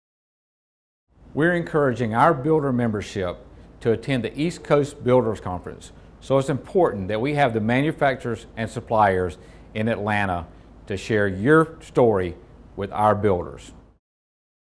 07 Lamar Smith v2
Lamar Smith with short message to manufacturers and suppliers